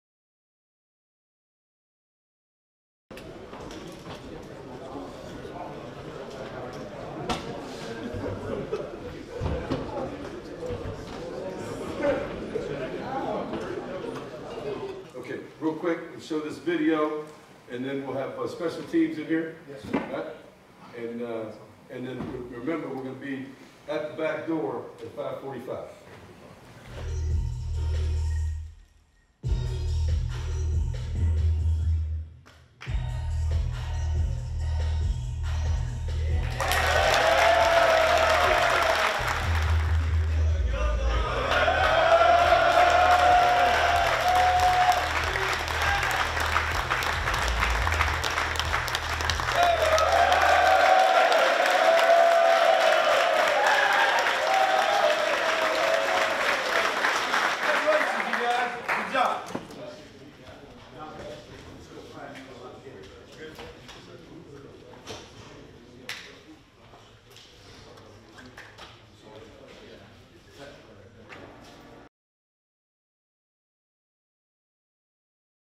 Texas A&M Football | Walk On Scholarships
Watch as Junior WR Boone Niederhofer and Senior RB Brice Dolezal are awarded full ride scholarships by Coach Kevin Sumlin.